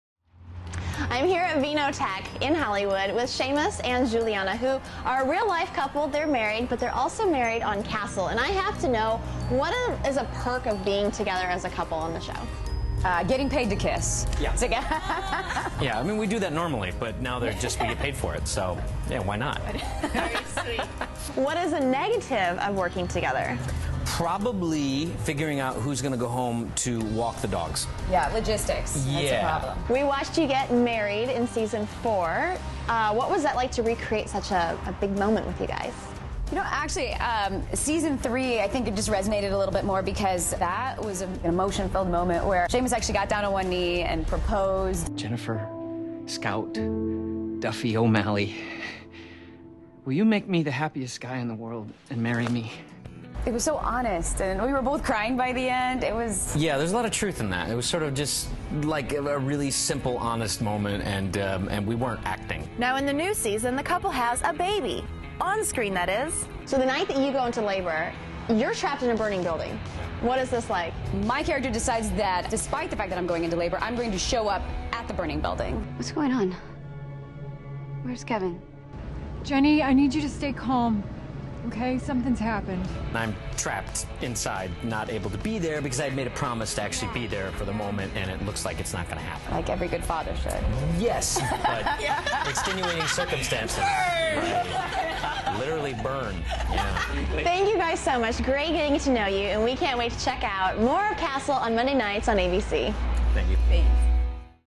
Castle Stars Seamus And Juliana Dever On Working As A Couple
They talk to our own Shandi Finnessey about being a married couple on AND off-screen.